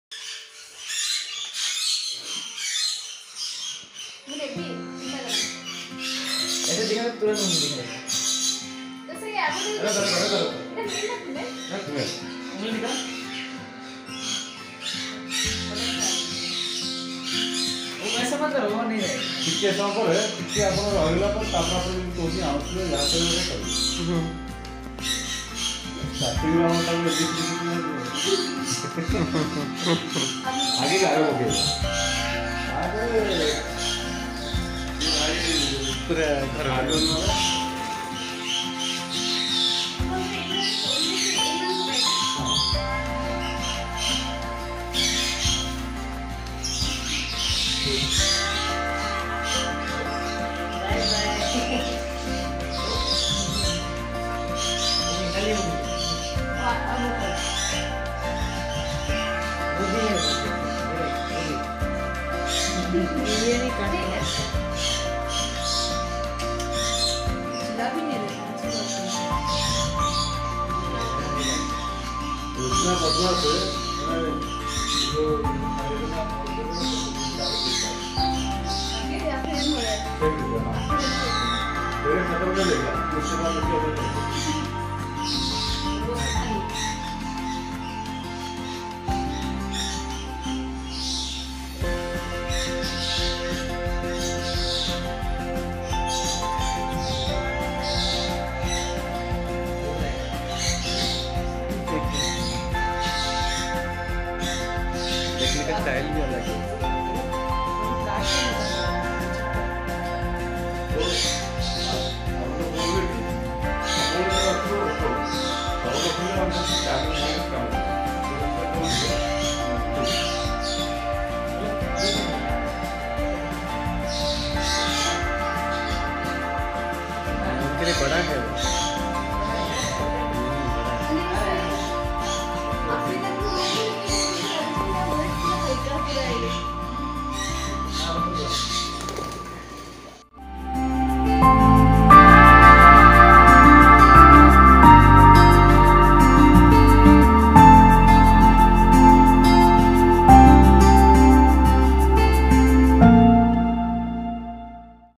Hand Tamed Chattering Lory @ Dhenkanal II Odisha II
The chattering lory is a forest-dwelling parrot endemic to North Maluku, Indonesia. It is considered vulnerable, the main threat being from trapping for the cage-bird trade. Now it is available with Mr. Girija in the Dhenkanal district of Odisha. Mr. Girija is the best in mutation of conure birds. This Chattering Lory is famous for its talking ability and its gorgeous looking.